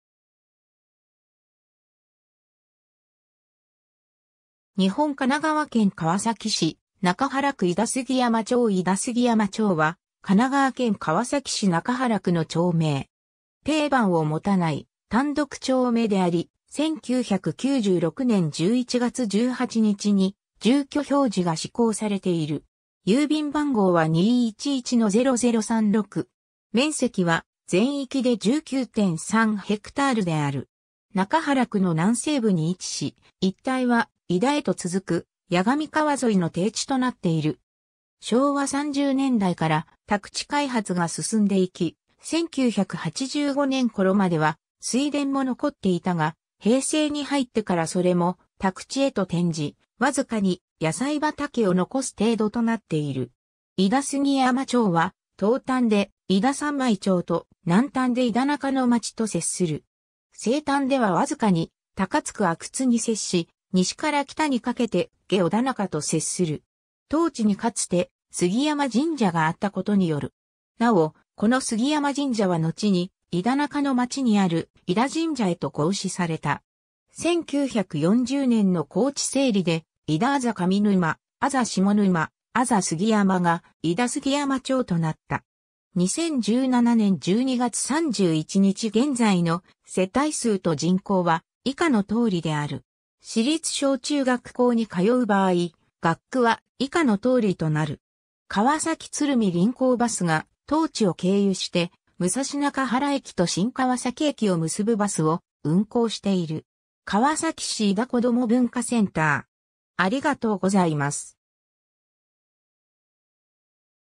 井田杉山町
中原区の町・字
日本 」 神奈川県 」 川崎市 」 中原区 」 井田杉山町 井田杉山町（いだすぎやまちょう）は、神奈川県川崎市中原区の町名。
丁番を持たない単独町名であり、1996年（平成8年）11月18日に住居表示が施行されている。
面積は全域で19.3haである。
 中原区の南西部に位置し、一帯は井田へと続く、矢上川沿いの低地となっている。
昭和30年代から宅地開発が進んでいき、1985年（昭和60年）頃までは水田も残っていたが、平成に入ってからそれも宅地へと転じ、わずかに野菜畑を残す程度となっている。
 井田杉山町は東端で井田三舞町と、南端で井田中ノ町と接する。
西端ではわずかに高津区明津に接し、西から北にかけて下小田中と接する（特記のない町域は中原区）。
 当地にかつて杉山神社があったことによる。
なお、この杉山神社はのちに、井田中ノ町にある井田神社へと合祀された。
 1940年（昭和15年）の耕地整理で、井田字上沼・字下沼・字杉山が井田杉山町となった。
 2017年（平成29年）12月31日現在の世帯数と人口は以下の通りである。
 川崎鶴見臨港バス（神明町営業所担当）が、当地を経由して武蔵中原駅と新川崎駅を結ぶバスを運行している。
 川崎市井田こども文化センター（2012年5月21日）